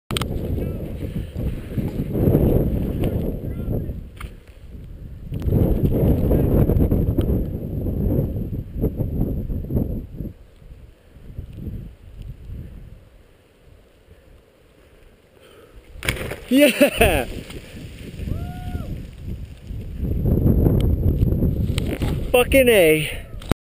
Tahoe Cliff Drop - Raw Footage
Cliff drop from a few years back at Tahoe.